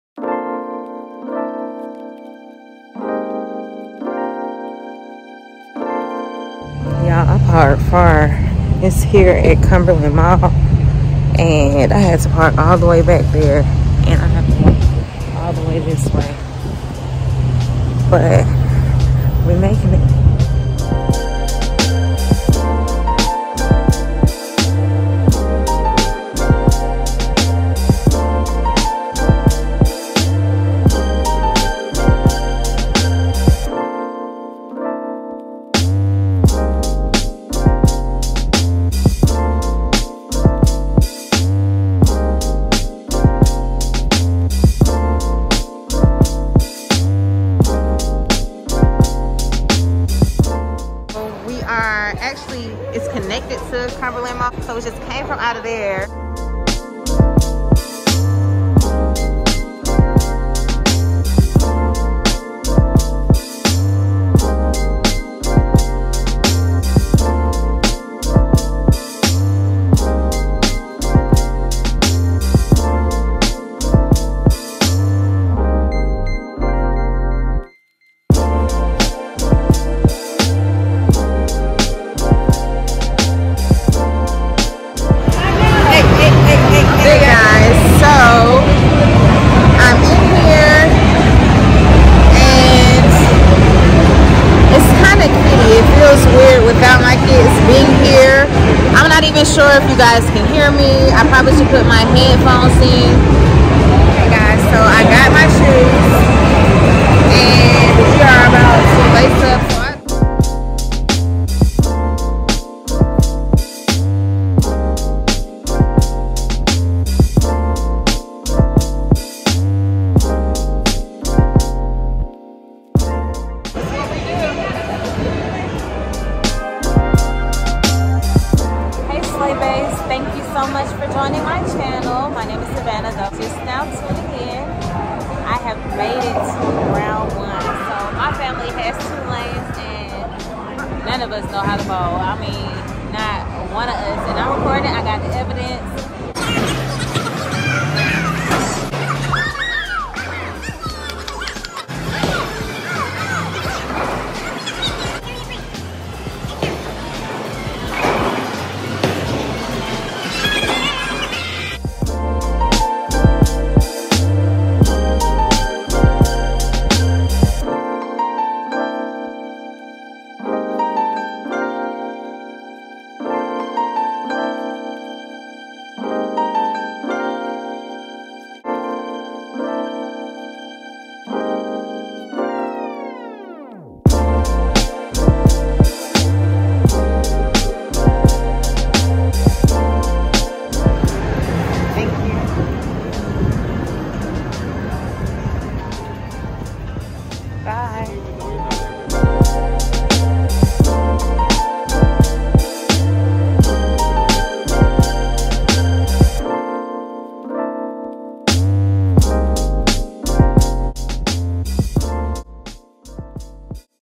Cousin’s Night - Round1 Bowling and Amusement
Hey SlayBaes! Here is a vlog of my night out with my cousins (multi generational) at Round1 Bowling and Amusement. We were so happy to get out of the house together after quarantine!  Enjoy!